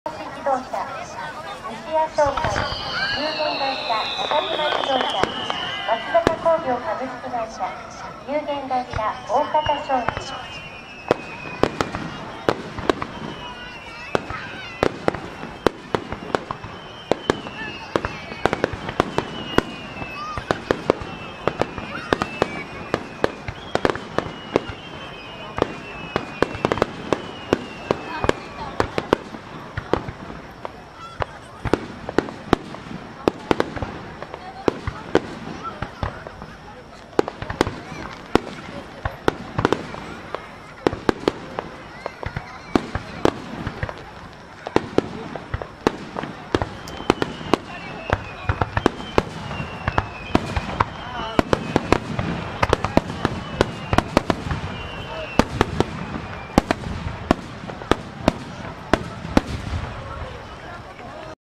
ひがしまつやま花火大会2019 スターマイン No48🔴Higashimatsuyama Japan Starmine
Time=令和元年８月24日 19時00分～20時30分　24th Aug 2019
Place=埼玉県東松山市都幾川リバーサイドパーク Saitama Pre Higashimatsuyama  shi tokigawa riverside park.